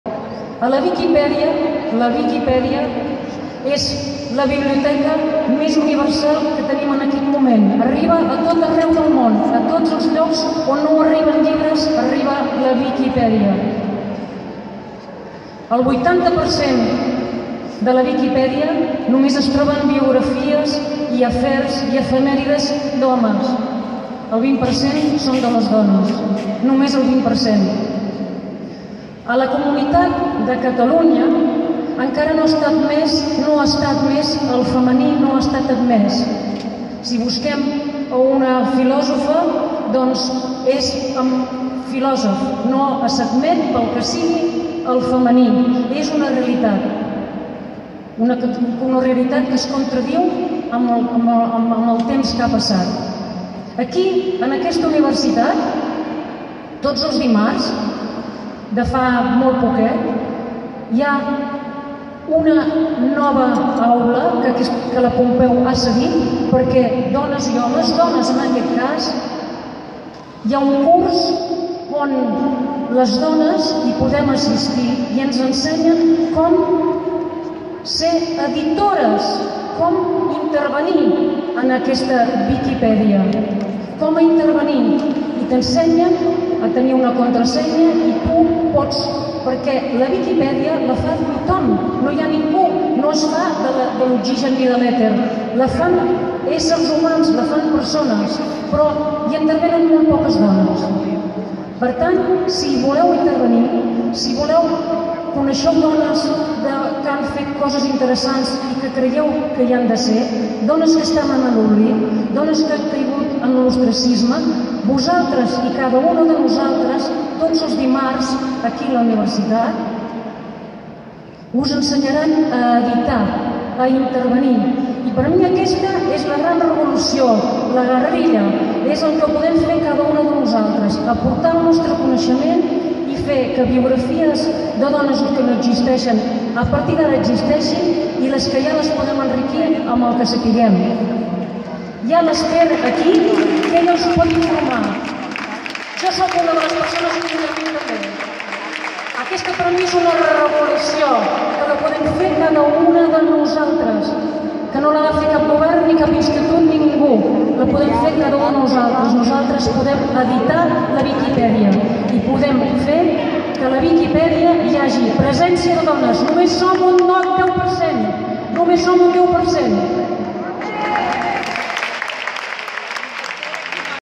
Viquidones Wikimujeres, Marina Rossell parla de la revolució d'editar Viquipèdia
Marina Rossell a la inauguració de les Jornades Feministes 2016 a la Universitat Pompeu Fabra de Barcelona